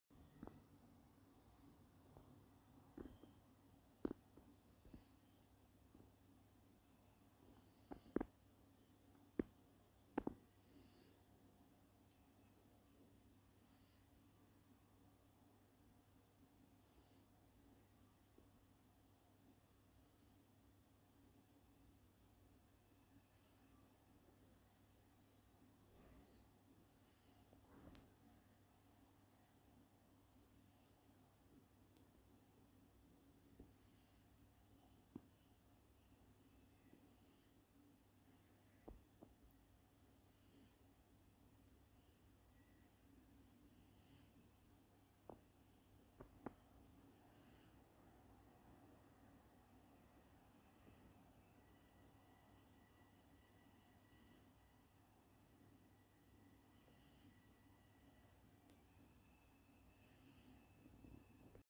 GSP German Shorthaired puppy Panzer daily nap... 💤🥰🐶
Thursday, 3/18/21
daily puppy nap